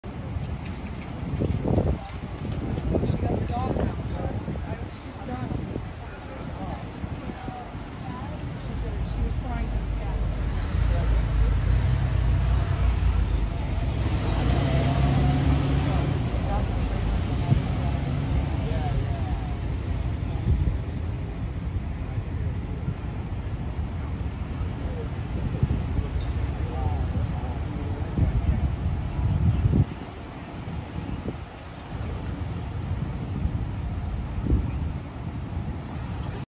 Idaho Youth Ranch Warehouse Fire
The Idaho Youth Ranch Warehouse caught fire Monday(7/18) afternoon.